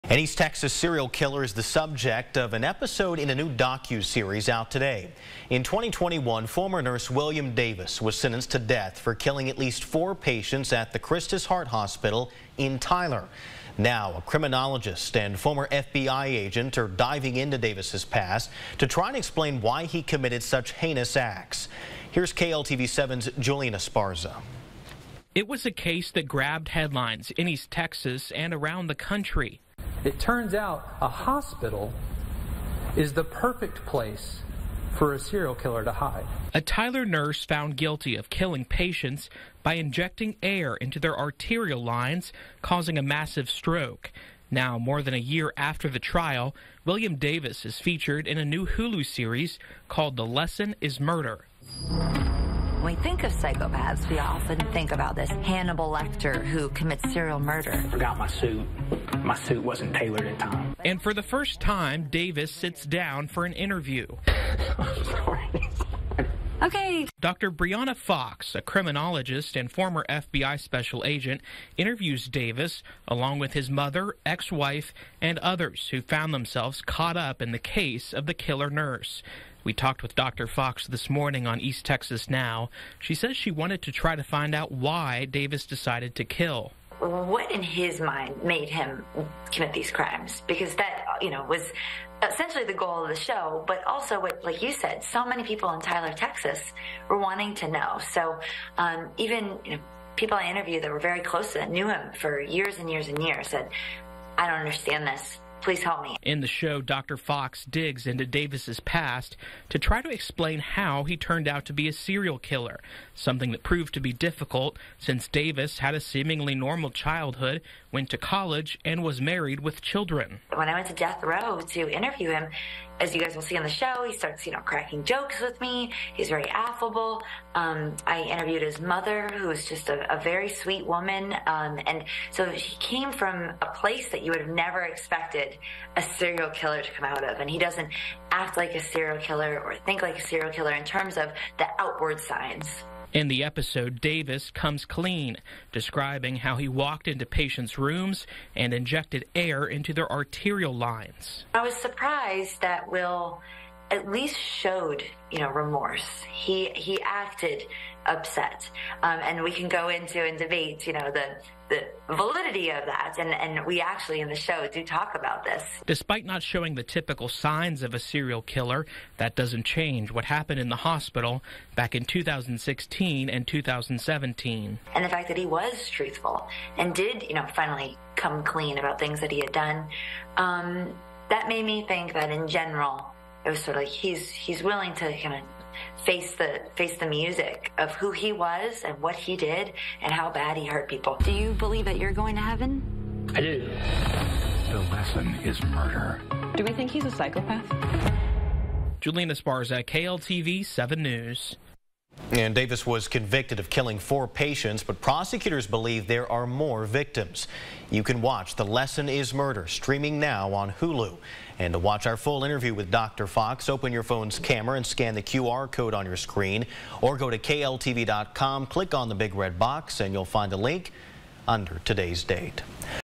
New docu-series examines case of Tyler hospital serial killer
A new TV series revisits the case of a Tyler hospital serial killer, William Davis. Criminologist Dr. Bryanna Fox speaks about her death row interview with him for the show.